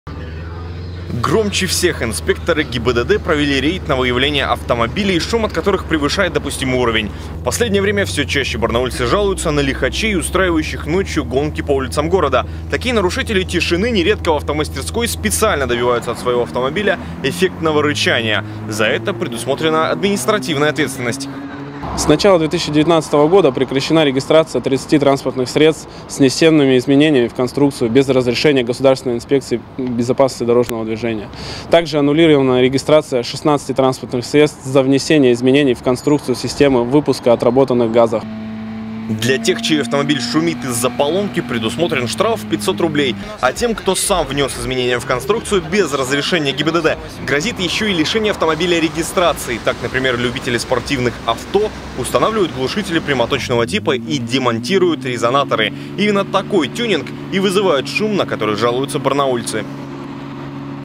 Любители «шумного» тюнинга рискуют остаться без авто
Громче всех. Инспекторы ГИБДД провели рейд на выявление автомобилей, которые превышает допустимый уровень шума. В последнее время все чаще барнаульцы жалуются на лихачей, которые ночью устраивают гонки по улицам города. Такие нарушители тишины нередко в автомастерской специально добиваются от своего автомобиля эффектного рычания – за это предусмотрена административная ответственность.